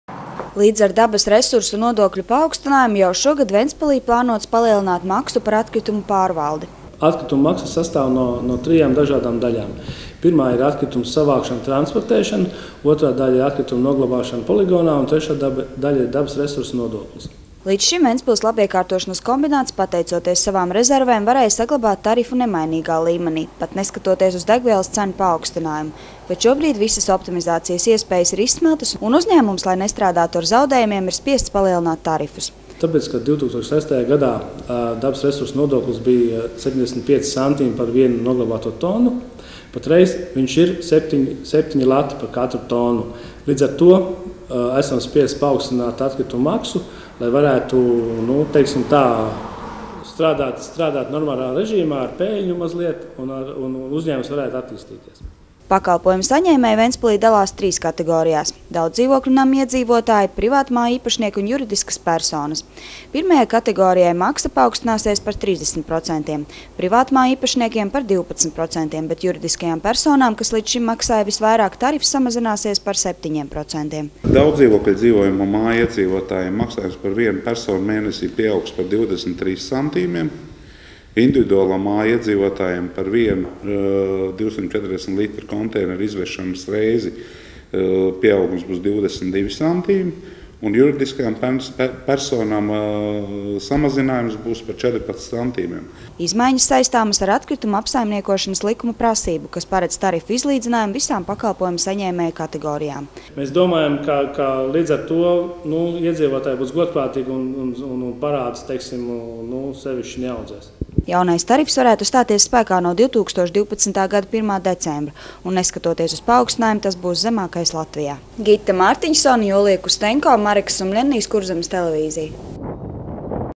Izmaiņas atkritumu apsaimniekošanas cenās
Dabas resursu nodokļa pieauguma dēļ šogad ventspilniekiem paredzēts paaugstināt maksu par atkritumu apsaimniekošanu. Tomēr tā joprojām būs viena no zemākajām Latvijā.